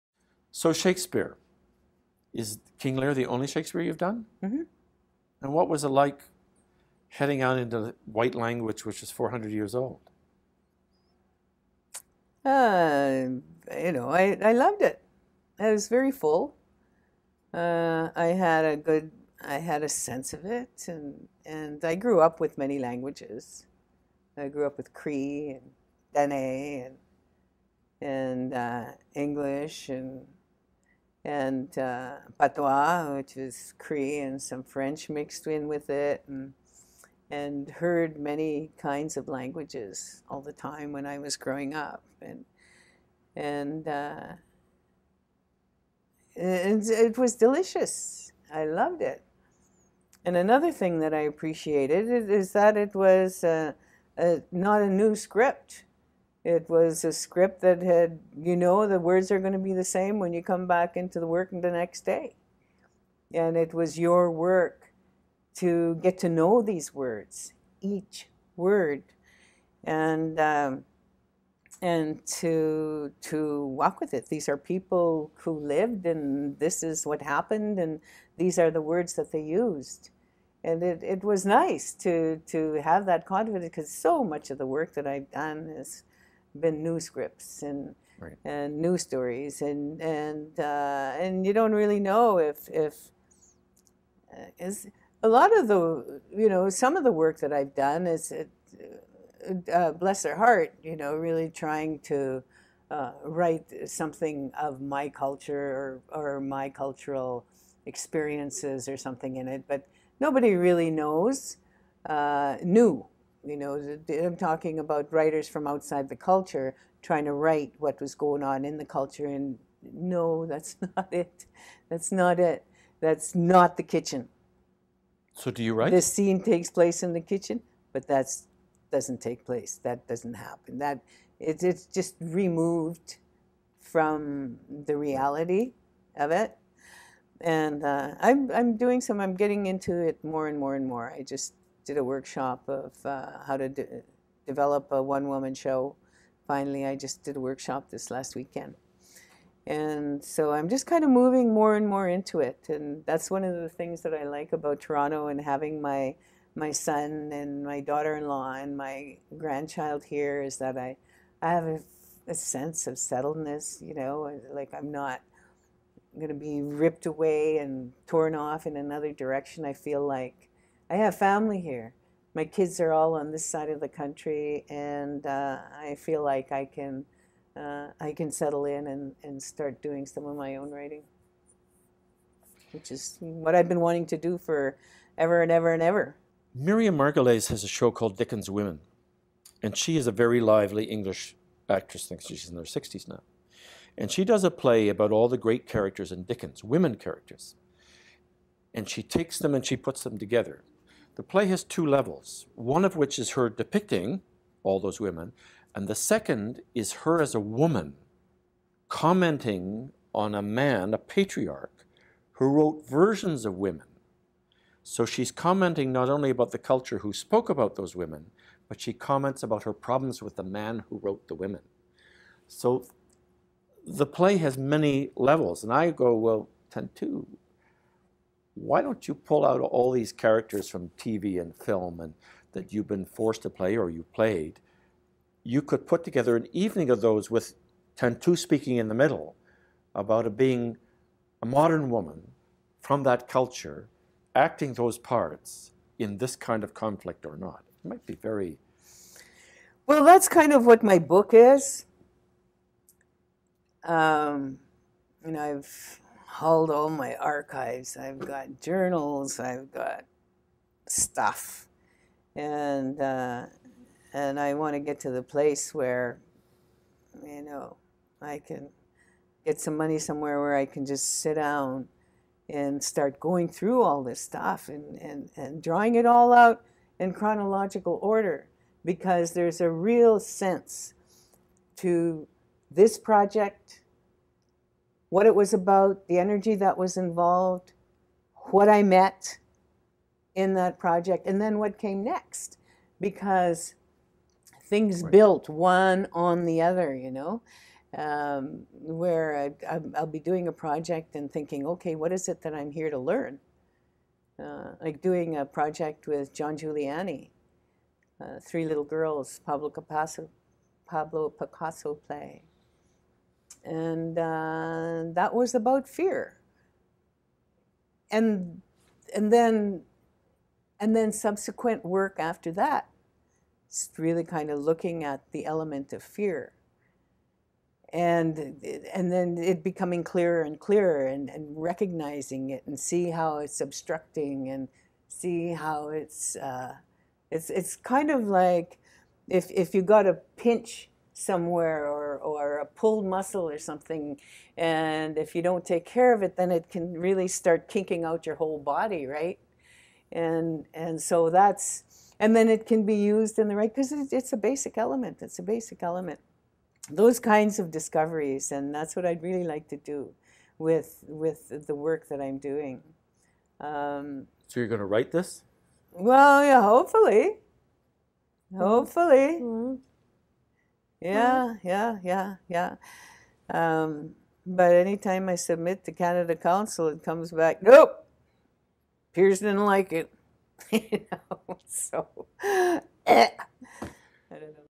Tantoo Cardinal on classical Shakespeare vs. new theatre works (Part 2 of 9)
Tantoo Cardinal discusses the differences and similarities between classical Shakespeare plays and new theatre works.

Interviewed by R.H. Thomson on December18th, 2012.
Filming location courtesy of Tarragon Theatre in Toronto, ON.